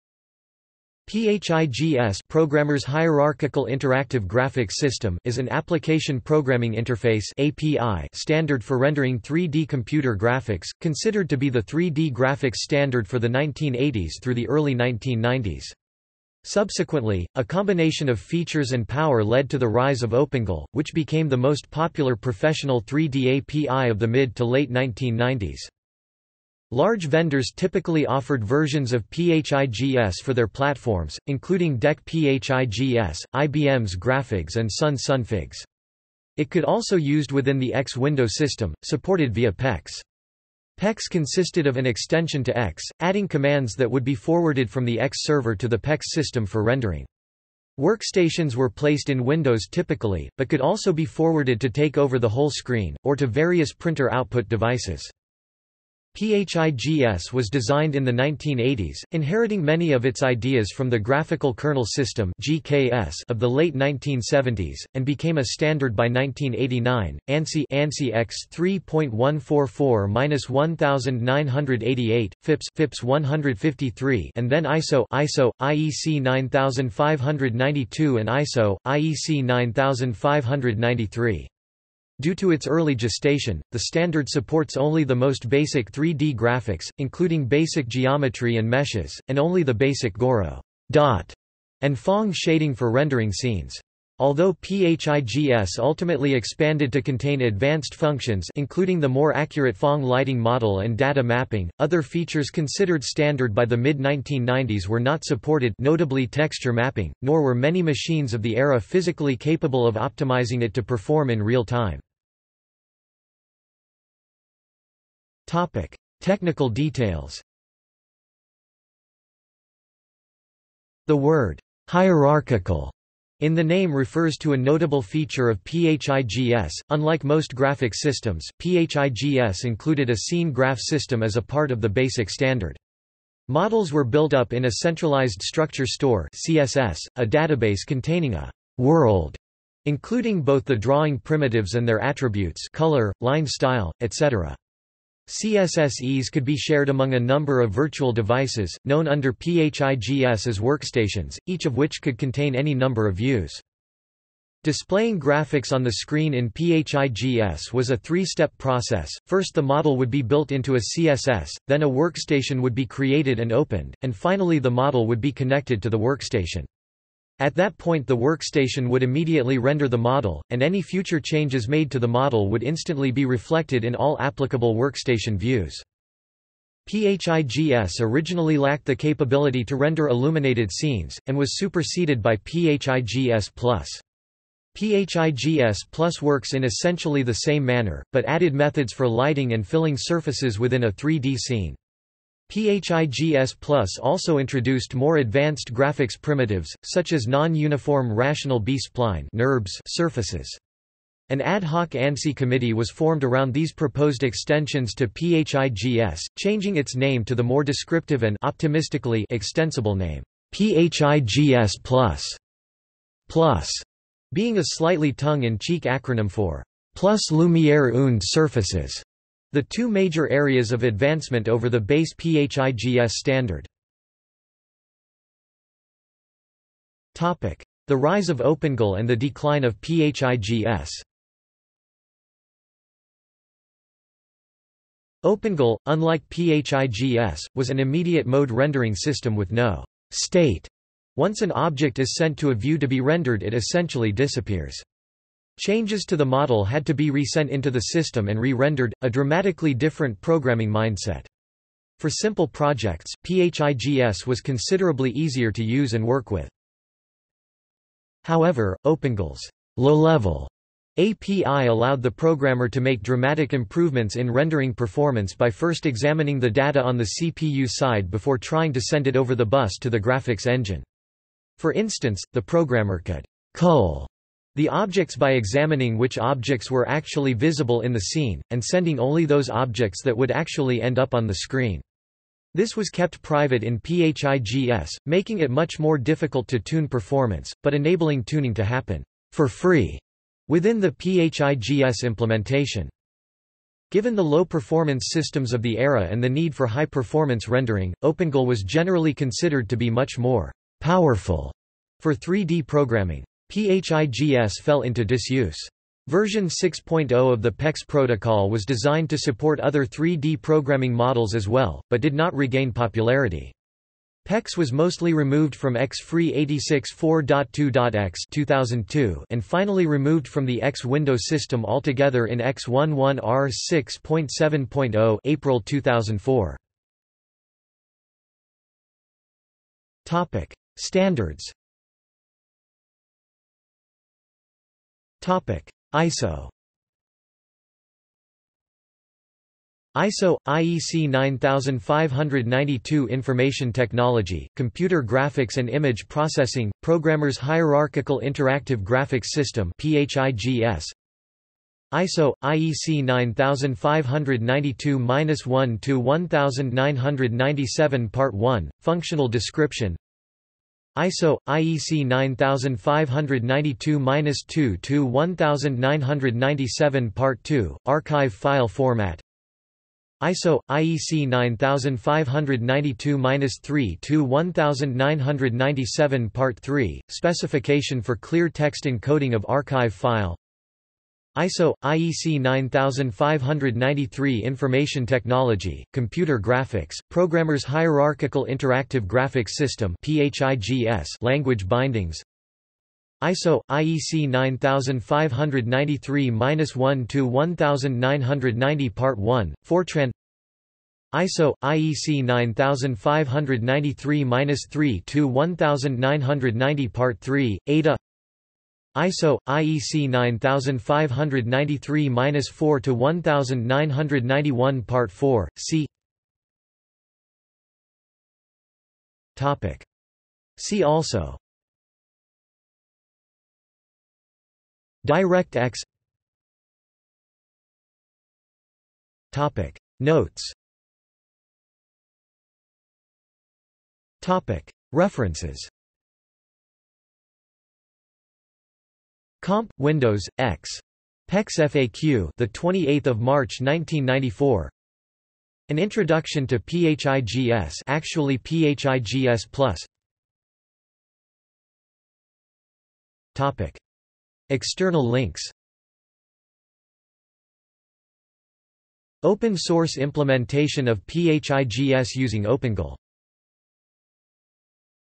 PHIGS | Wikipedia audio article
This is an audio version of the Wikipedia Article:
PHIGS

- Socrates

SUMMARY
PHIGS (Programmer's Hierarchical Interactive Graphics System) is an application programming interface (API) standard for rendering 3D computer graphics, considered to be the 3D graphics standard for the 1980s through the early 1990s. Subsequently, a combination of features and power led to the rise of OpenGL, which became the most popular professional 3D API of the mid to late 1990s.
Large vendors typically offered versions of PHIGS for their platforms, including DEC PHIGS, IBM's graPHIGS and Sun's SunPHIGS. It could also used within the X Window System, supported via PEX. PEX consisted of an extension to X, adding commands that would be forwarded from the X server to the PEX system for rendering. Workstations were placed in windows typically, but could also be forwarded to take over the whole screen, or to various printer-output devices.
PHIGS was designed in the 1980s, inheriting many of its ideas from the Graphical Kernel System (GKS) of the late 1970s, and became a standard by 1989: ANSI (ANSI X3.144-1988), FIPS (FIPS 153) and then ISO (ISO/IEC 9592 and ISO/IEC 9593). Due to its early gestation, the standard supports only the most basic 3D graphics, including basic geometry and meshes, and only the basic Gouraud, "Dot", and Phong shading for rendering scenes. Although PHIGS ultimately expanded to contain advanced functions (including the more accurate Phong lighting model and Data Mapping), other features considered standard by the mid-1990s were not supported (notably texture mapping), nor were many machines of the era physically capable of optimizing it to perform in real time.